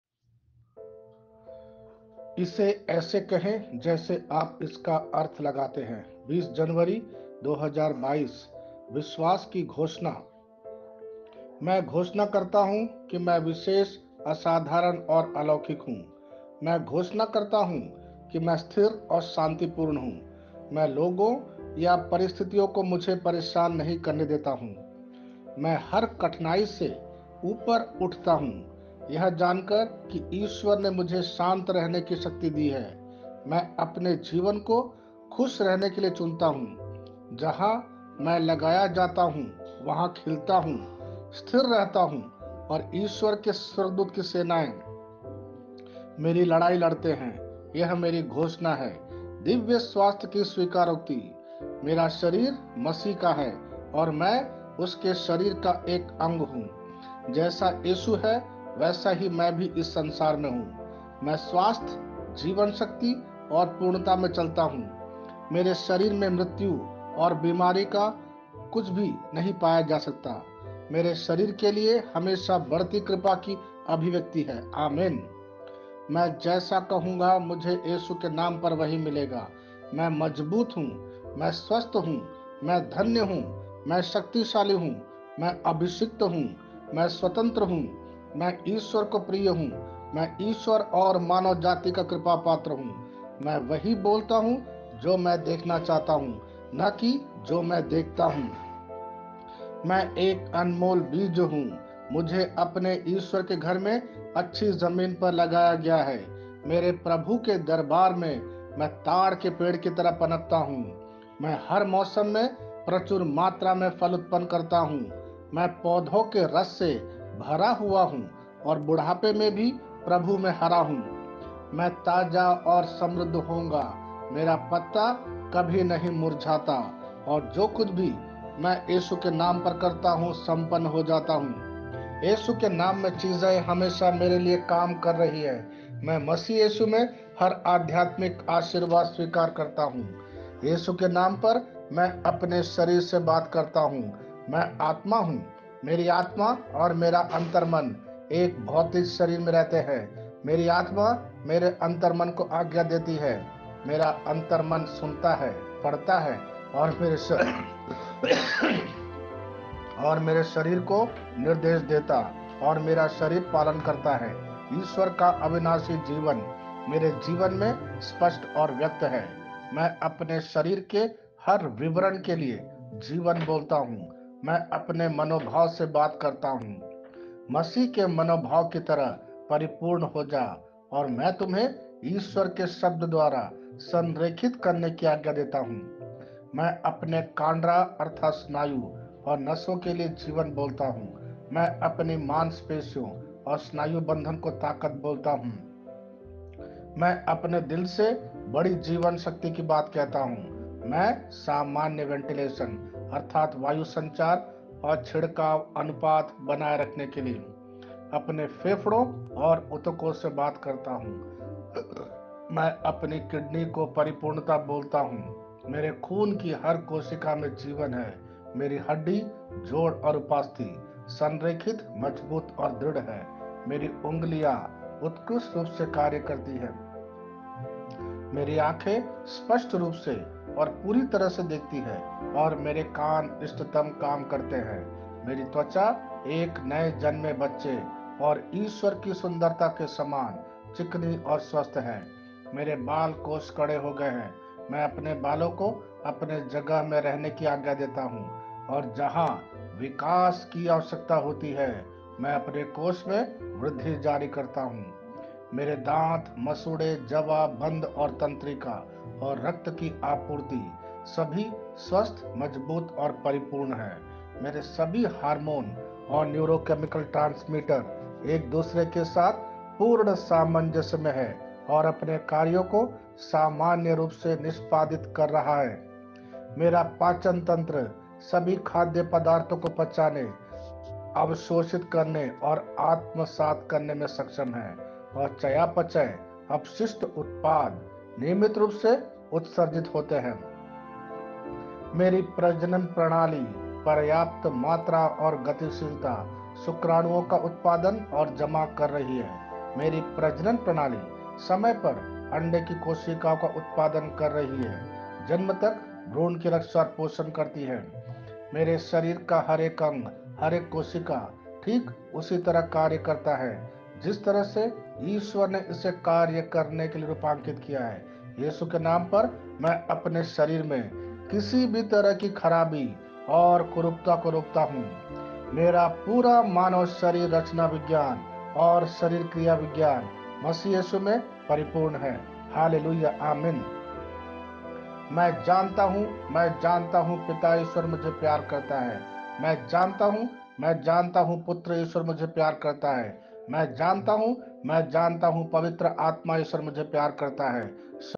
20th January 2022 Say It like You Mean It (Audio in Hindi)
इसे ऐसे कहे जैसे आप इसका अर्थ लगाते है।
20 जनवरी 2022

विश्वास की घोषणा
 मैं घोषणा करता हूं कि मैं विशेष, असाधारण और अलौकिक हूं।
 मैं घोषणा करता हूं कि मैं स्थिर और शांतिपूर्ण हूं.। मैं लोगों या परिस्थितियों को मुझे परेशान नहीं करने देता हूं।
 मैं हर कठिनाई से ऊपर उठता हूं, यह जानकर कि ईश्वर ने मुझे शांत रहने की शक्ति दी है। मैं अपने जीवन को खुश रहने के लिए चुनता हूं, जहां मैं लगाया जाता हूं, वहां खिलता हूं, स्थिर रहता हूं और ईश्वर के स्वर्गदूत की सेनाएं  मेरी लड़ाई लड़ते है। यह मेरी घोषणा है।

दिव्य स्वास्थ्य की स्वीकारोक्ति
मेरा शरीर मसीह का है और मैं उसके शरीर का एक अंग हूँ। जैसा यीशु है, वैसा ही मैं भी इस संसार में हूँ। मैं स्वास्थ्य, जीवन शक्ति और पूर्णता में चलता हूं। मेरे शरीर में मृत्यु और बीमारी का कुछ भी नहीं पाया जा सकता है, मेरे शरीर के लिए हमेशा- बढ़ती कृपा की अभिव्यक्ति है आमेन

मै जैसा  कहूँगा मुझे यीशु के नाम पर  वही मिलेगा

मैं मजबूत हूँ येशु के नाम पर।

मैं स्वस्थ हूं येशु के नाम पर।

मैं धन्य हूँ येशु के नाम पर।

मैं शक्तिशाली हूँ येशु के नाम पर।

मैं अभिशिक्त हुं येशु के नाम पर।

मैं आज़ाद हूं येशु के नाम पर।
मैं ईश्वर को प्रिय  हूँ येशु के नाम पर।

मैं ईश्वरऔर मानव जाति  का कृपापत्र  हूँ येशु के नाम पर।

मैं वही बोलता हूं जो मैं देखना चाहता हूं न की जो मैं देखता हूं येशु के नाम पर।

मैं एक अनमोल बीज हूं येशु के नाम पर।

मुझे अपने ईश्वर के घर में अच्छी जमीन पर लगाया गया है।

मेरे प्रभु के दरबार में मैं ताड़ के पेड़ की तरह पनपता हूं।

मैं हर मौसम में प्रचुर मात्रा में फल उत्पन्न करता हूं।

मैं पौधों के रस से भरा हुआ हूं| और बुढ़ापे में भी
प्रभु में हरा हूं।
मैं ताजा
 और समृद्ध होऊंगा।

मेरा पत्ता कभी नहीं मुरझाता।
और जो कुछ भी मैं येशु के नाम पर करता हूं संपन्न हो जाता हूं।

येशु के नाम में चीजें हमेशा मेरे लिए काम कर रही हैं।

मैं मसीह येशु में हर आध्यात्मिक आशीर्वाद स्वीकार करता हूँ।

येशु के नाम पर मैं अपने शरीर से बात करता हूं।
मैं आत्मा हूं
मेरी आत्मा और मेरा  अंतर्मन  एक भौतिक शरीर में रहते हैं।मेरी आत्मा मेरी अंतर्मन को* आज्ञा देती है। *मेरा अंतर्मन सुनता है, पढ़ता है और मेरे शरीर को निर्देश देता और मेरा शरीर  पालन करता है।

ईश्वर का अविनाशी जीवन मेरे जीवन में स्पष्ट और व्यक्त है।
 मैं अपने शरीर के हर विवरण के लिए जीवन बोलता हूं।
मैं अपने मनोभाव से बात करता हुं, मसीह के मनोभाव की तरह परिपूर्ण हो जा। और मैं तुम्हें  ईश्वर के शब्द द्वारा संरेखित करने की आज्ञा देता हूं।
मैं अपने कांडरा (स्नायु) और नसों के लिए जीवन बोलता हूं।
मैं अपनी मांसपेशियों और स्नायुबंधन को ताकत बोलता हूं।
मैं अपने दिल से बडी जीवन शक्ति की बात कहता हूं।
मैं सामान्य वेंटिलेशन (वायु संचार )और छिड़काव अनुपात बनाए रखने के लिए अपने फेफड़ों और ऊतकों से बात करता हूं।
 मैं अपनी  किड़नी को परिपूर्णता बोलता हूं।
 मेरे खून की हर कोशिका में जीवन है।
मेरी हड्डी, जोड़ और उपास्थि ,संरेखित, मजबूत और दृढ़ हैं।
मेरी उंगलियां उत्कृष्ट रूप से कार्य करती हैं। मेरी आँखें स्पष्ट रूप से और पूरी तरह से देखती हैं । और मेरे कान इष्टतम काम करते हैं।

मेरी त्वचा एक नए जन्मे बच्चे और ईश्वर  की सुंदरता के समान चिकनी, स्वस्थ है।

मेरे बाल कोश कड़े हो गए हैं। मैं अपने बालों को अपने जगह में रहने की आज्ञा देता हूं और जहां विकास की आवश्यकता होती है, मैं अपने कोश में वृद्धि जारी करता हूं।
मेरे दांत, मसूड़े, जवा, बंध और तंत्रिका और रक्त की आपूर्ति सभी स्वस्थ, मजबूत और परिपूर्ण हैं।

मेरे सभी हार्मोन और न्यूरोकेमिकल ट्रांसमीटर एक दूसरे के साथ पूर्ण सामंजस्य में हैं।और अपने कार्यों को सामान्य रूप से निष्पादित कर रहा है।

मेरा पाचन तंत्र सभी खाद्य पदार्थों को पचाने, अवशोषित करने और आत्मसात करने में सक्षम है। और चयापचय अपशिष्ट उत्पाद नियमित रूप से उत्सर्जित होते हैं।

🙎🏻‍♂️मेरी  प्रजनन प्रणाली पर्याप्त मात्रा और गतिशीलता शुक्राणुओं का उत्पादन और जमा कर रही है/
🙍🏻‍♀️मेरी प्रजनन प्रणाली समय पर अंडे की * कोशिकाओं का उत्पादन कर रही है, जन्म तक भ्रूण की रक्षा और पोषण करती है।

 मेरे शरीर का  हर एक अंग ,हर एक कोशिका ठीक उसी तरह कार्य करता है जिस तरह से  ईश्वर ने इसे कार्य करने के लिए रूपांकित किया है।
येशु के  नाम पर मैं अपने शरीर में किसी भी तरह की खराबी और कुरूपता को रोकता  हुं। *
मेरा पूरा मानव शरीर रचना विज्ञान और शरीर क्रिया विज्ञान मसीह यीशु में परिपूर्ण है।
 अल्लेलूया आमेन*

मैं जानता हूं, मैं जानता हुं पिता ईश्वर मुझे प्यार करता है।
मैं जानता हुं  मैं जानता हुं पुत्र ईश्वर मुझे प्यार करता हैं।
मैं जानता हुं  मैं जानता हुं पवित्र आत्मा ईश्वर मुझे प्यार करता हैं।

 शालोम